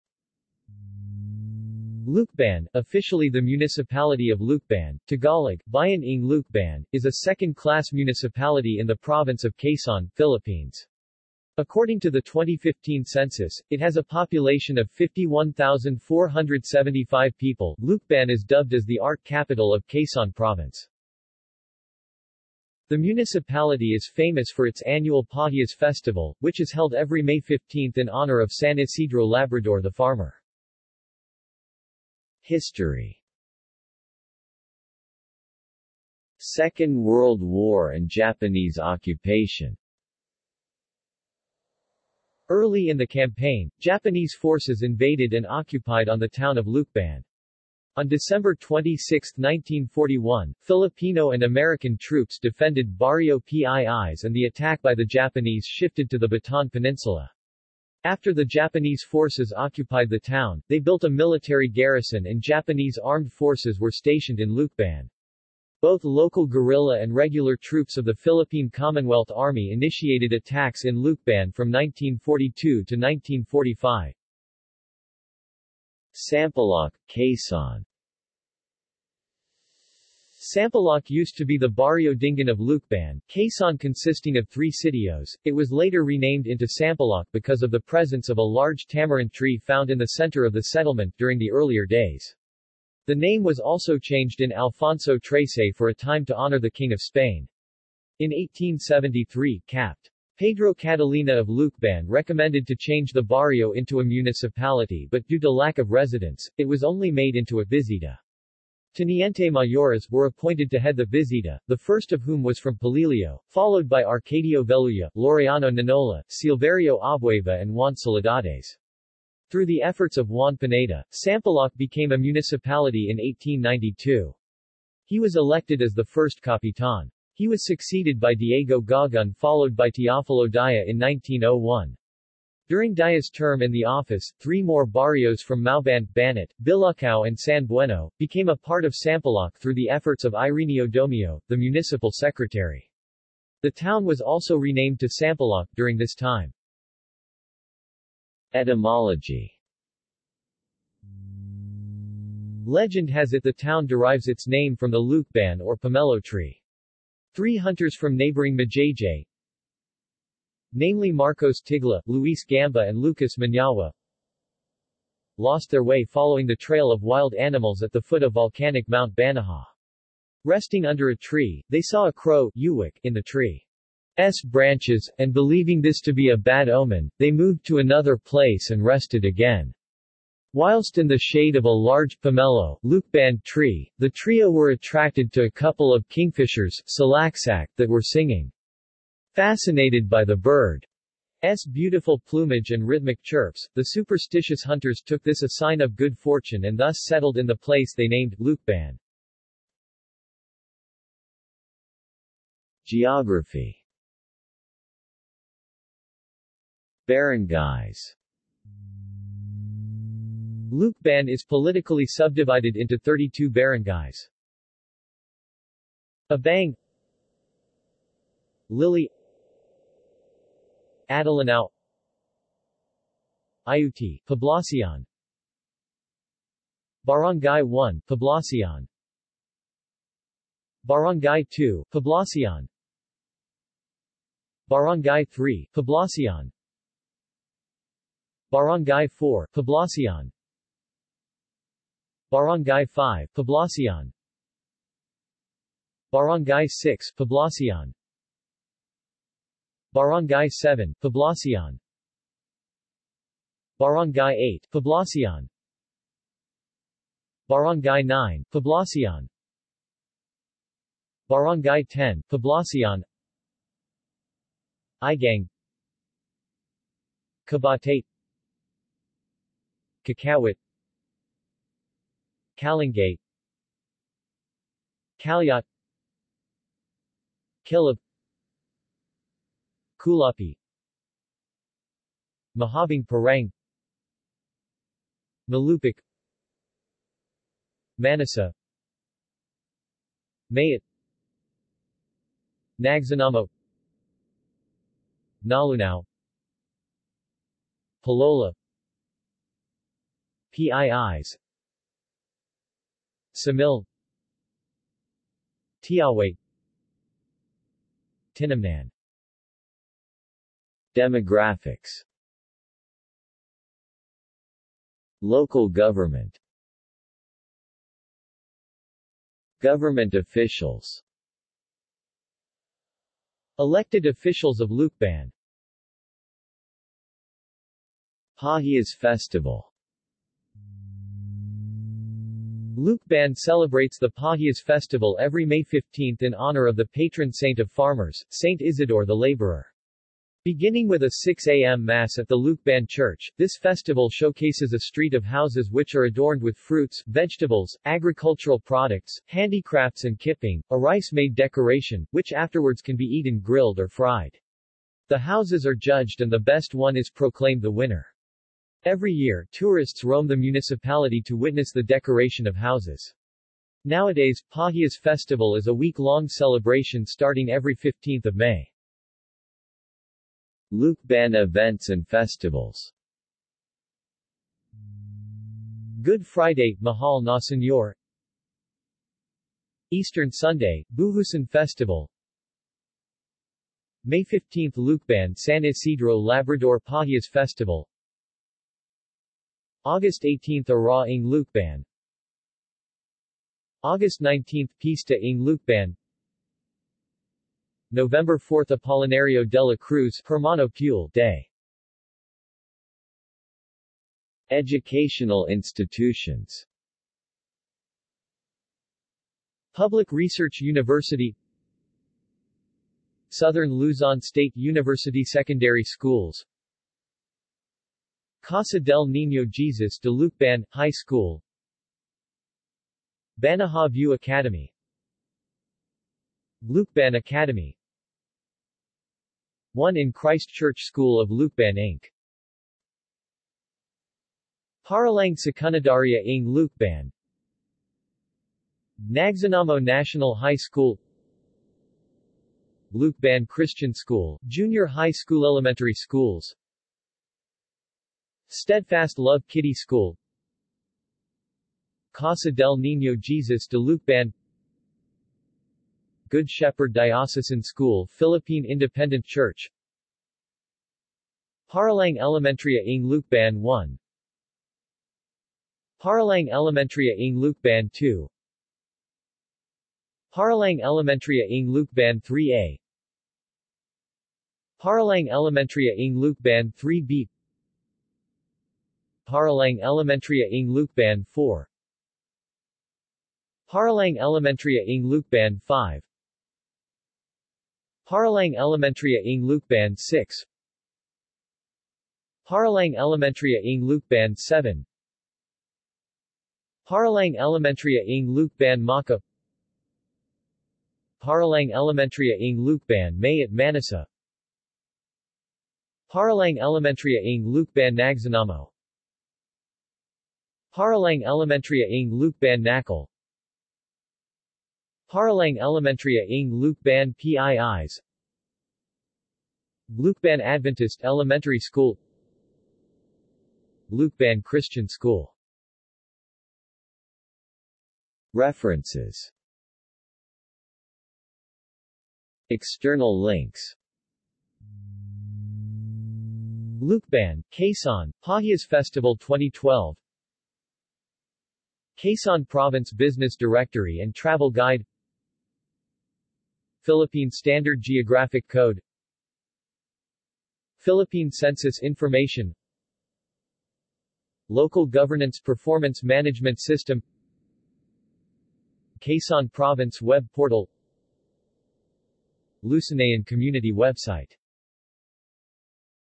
Lucban, Quezon | Wikipedia audio article
This is an audio version of the Wikipedia Article:
Lucban, Quezon

SUMMARY
Lucban, officially the Municipality of Lucban, (Tagalog: Bayan ng Lukban), is a 2nd class municipality in the province of Quezon, Philippines. According to the 2015 census, it has a population of 51,475 people.Lucban is dubbed as the Art Capital of Quezon province.
The municipality is famous for its annual Pahiyas Festival, which is held every May 15 in honor of San Isidro Labrador the Farmer.